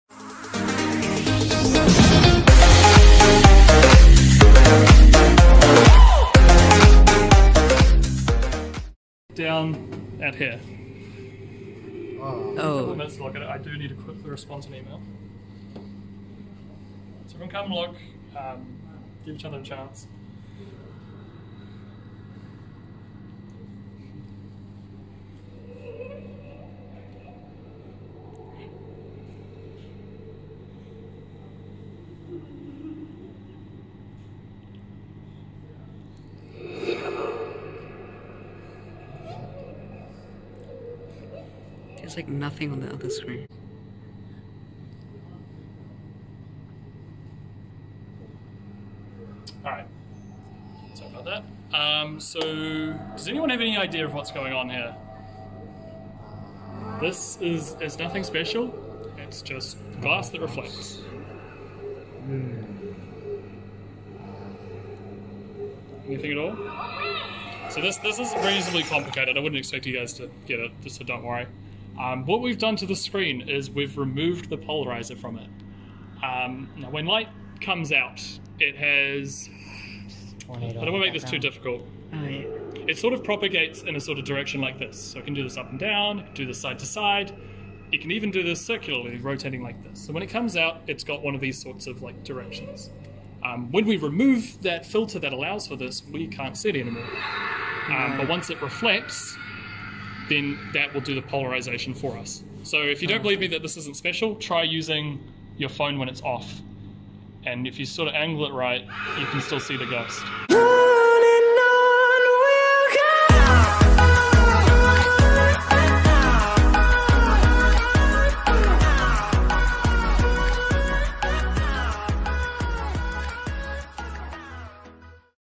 Trapped Ghost at the University of Auckland
In this episode, we are at The University of Auckland doing experiments related to physics during the school physics trip to UoA. We are looking at a trapped ghost in this video which was interesting!!